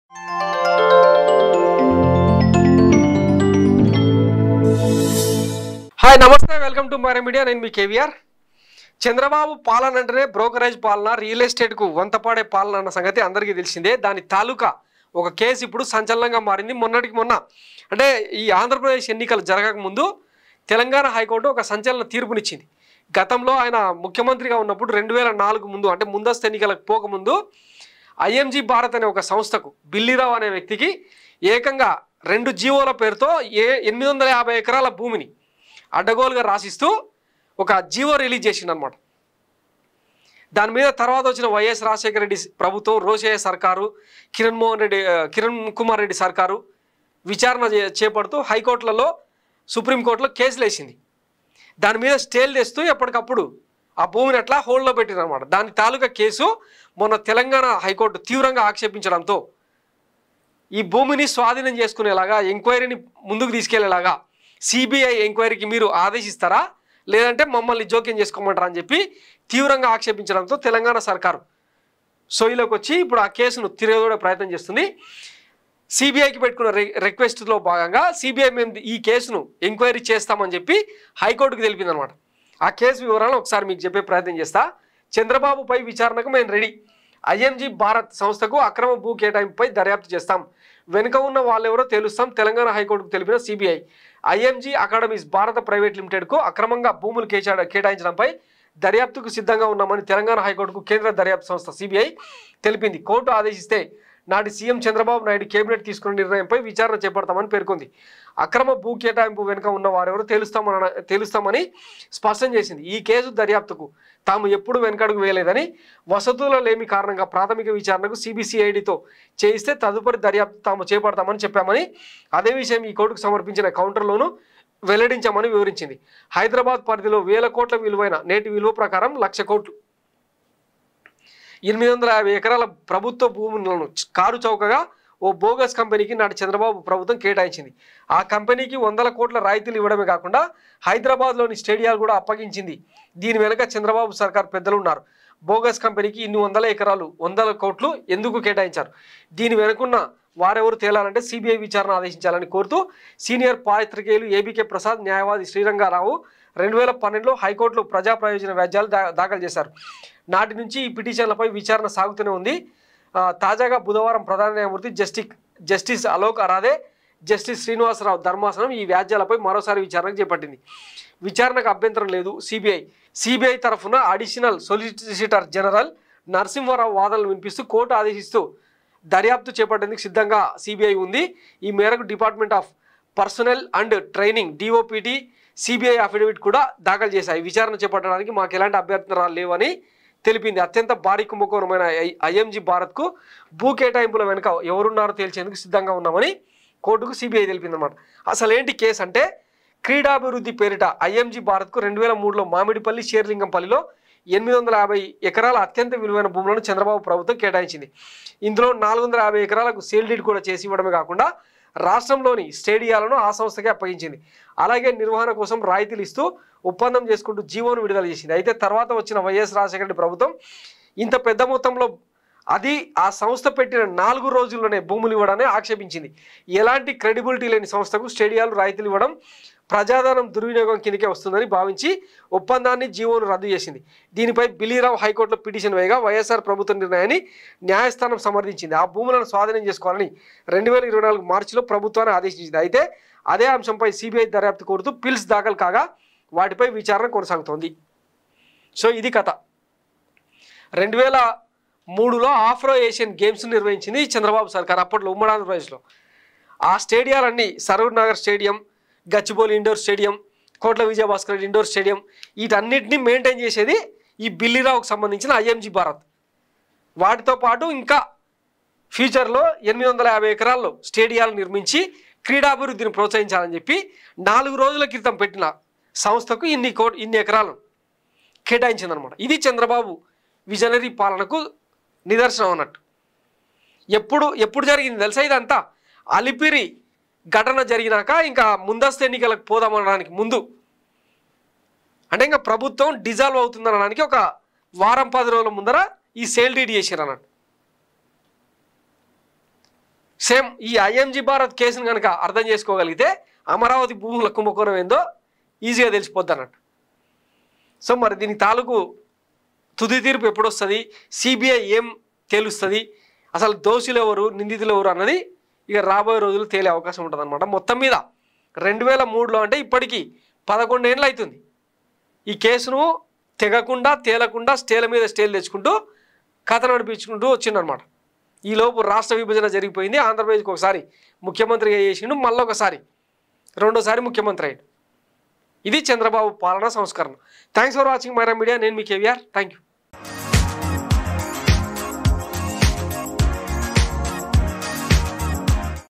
ఐఎంజీ భారత్ ఎంక్వైరీకి మేం రెడీ...! KVR Analysis About Img Bharat Scam | CBI Cases On Chandrababu
Watch :: ఐఎంజీ భారత్ ఎంక్వైరీకి మేం రెడీ...! KVR Analysis About Img Bharat Scam | CBI Cases On Chandrababu | @MyraMedia